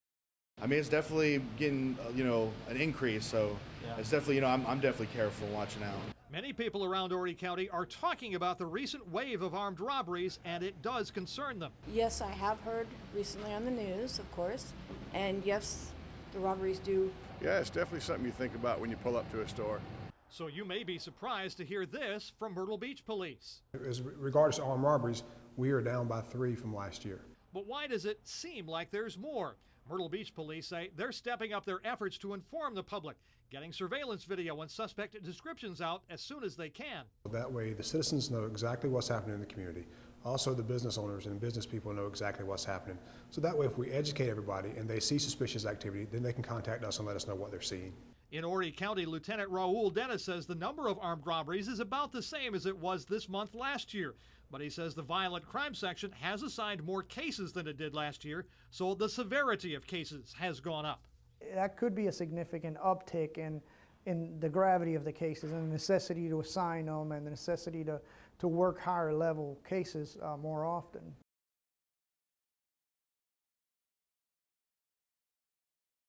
Public perception of armed robberies not the same as reality
According to the two largest police departments in our area, the number of armed robberies is either about the same or lower than last year.